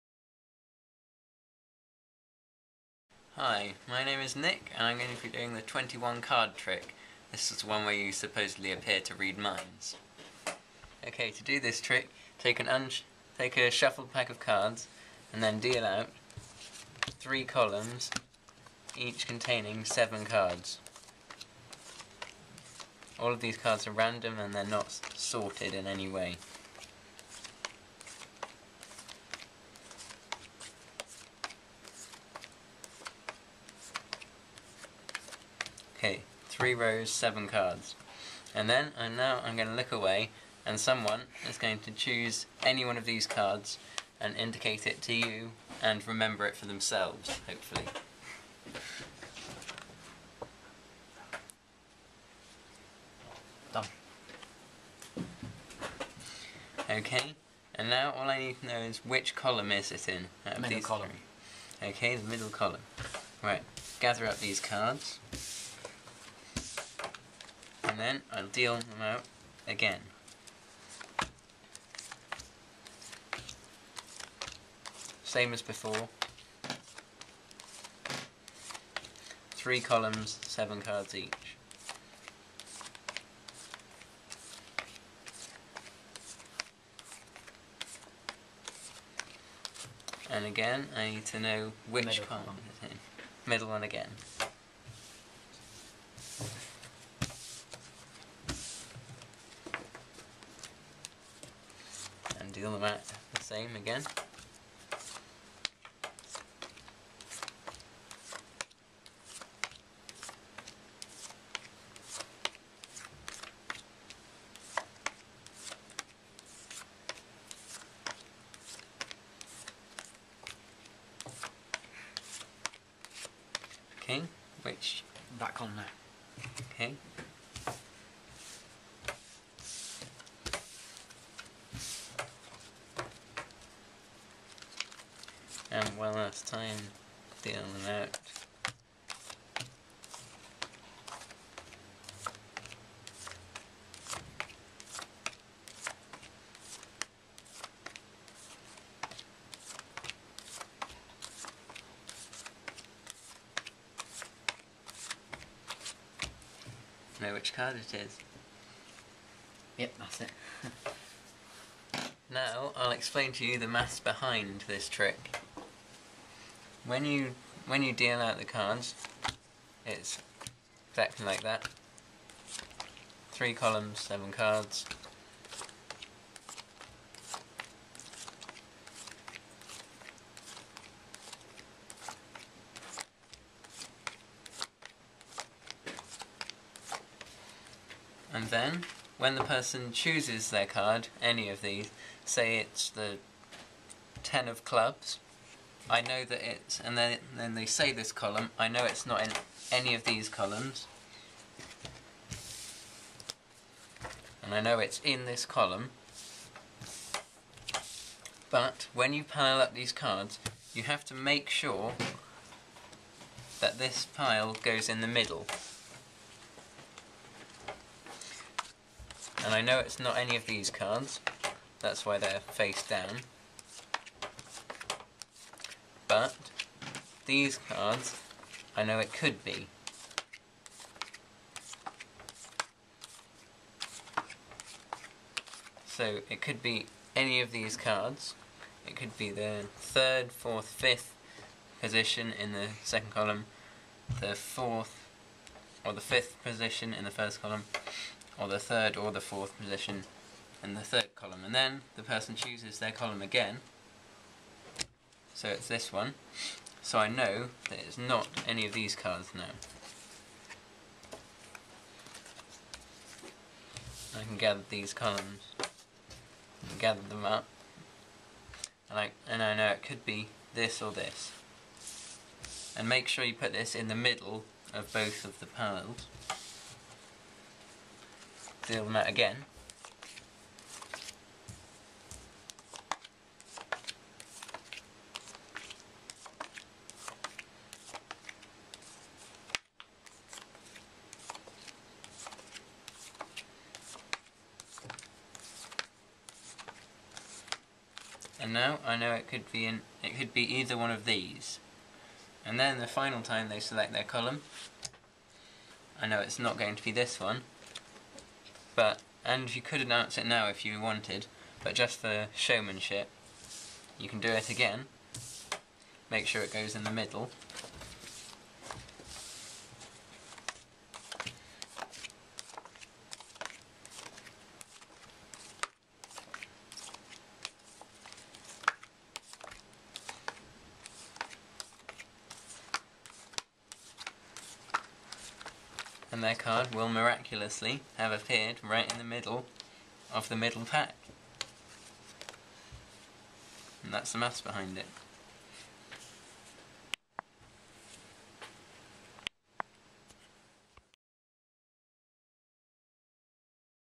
The 21 Card trick
a Card trick with mathematical explanation recorded at the University of Plymouth